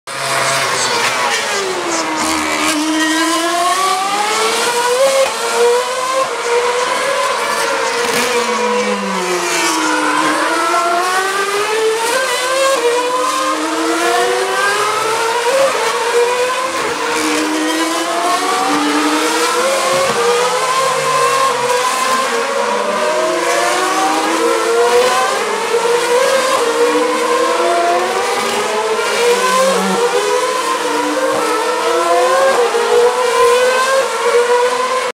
2013 Singapore Grand Prix Turn 3
Comparison of different drivers exiting turn 3